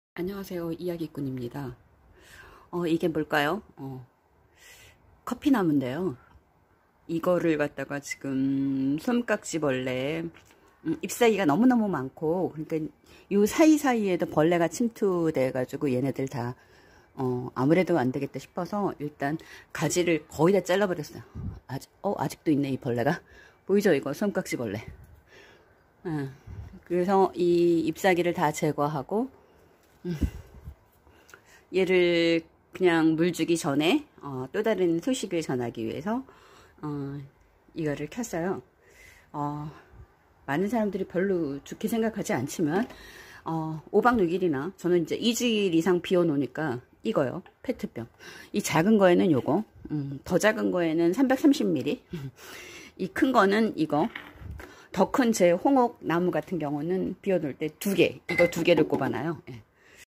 솜깍지벌레의 피해입은 커피나무
커피나무를 거의 다 가지를 잘라버렸다.